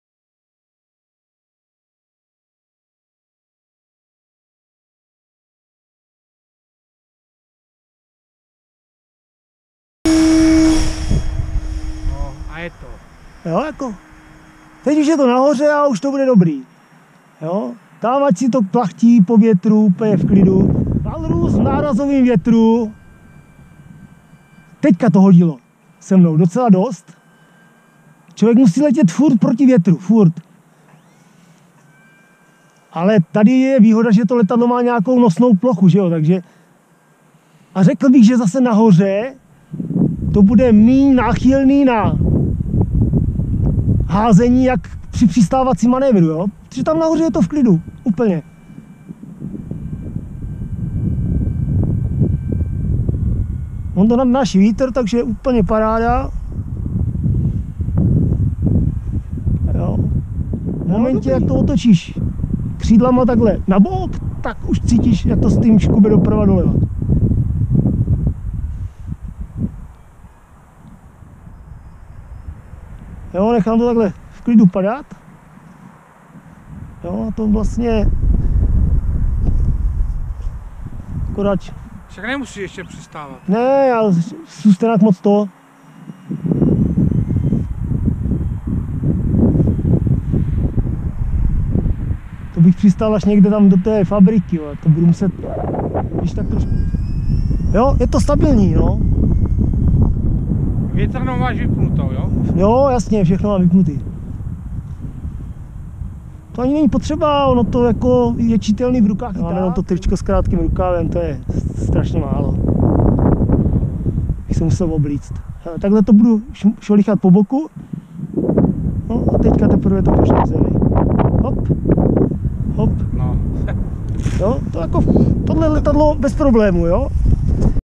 Night Walrus test v poryvu větru
Žádná stabilizace, vítr kolem 30km/h v nárazech i více. Walrus překvapuje.